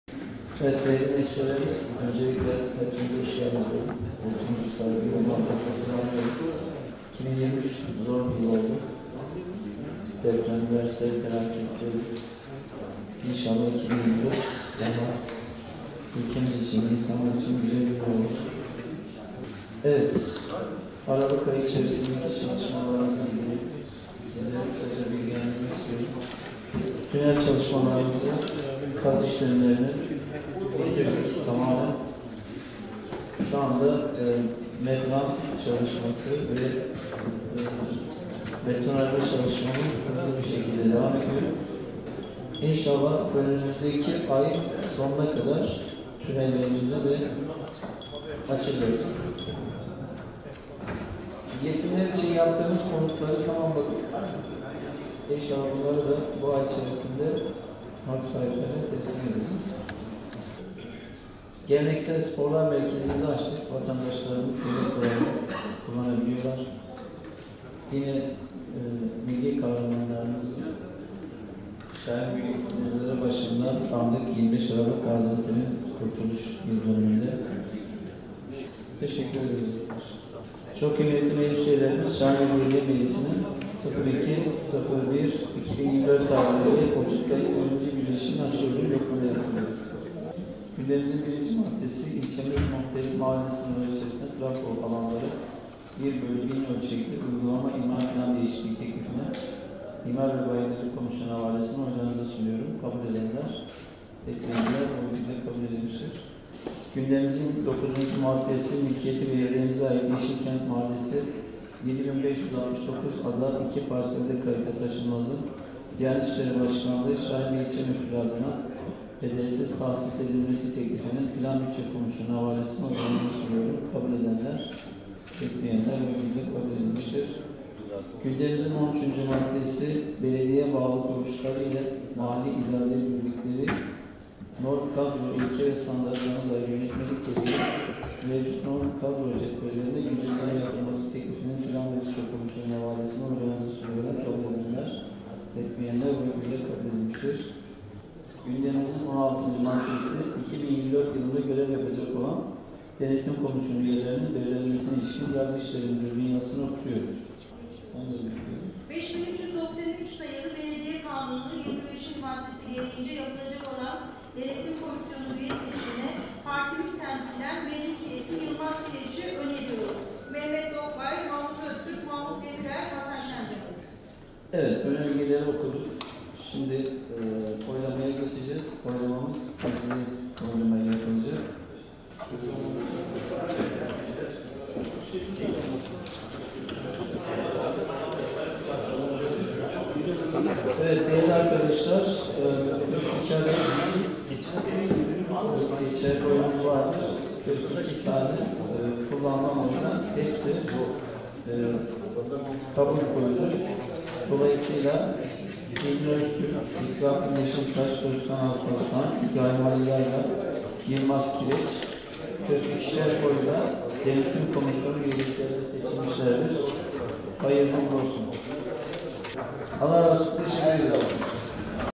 Şahinbey’de Yeni Yılın İlk Meclis Toplantısı Yapıldı
2024 yılının ilk meclis toplantısının birinci oturumu Şahinbey Belediyesi Meclis Başkanı Mehmet Tahmazoğlu Başkanlığında gerçekleştirildi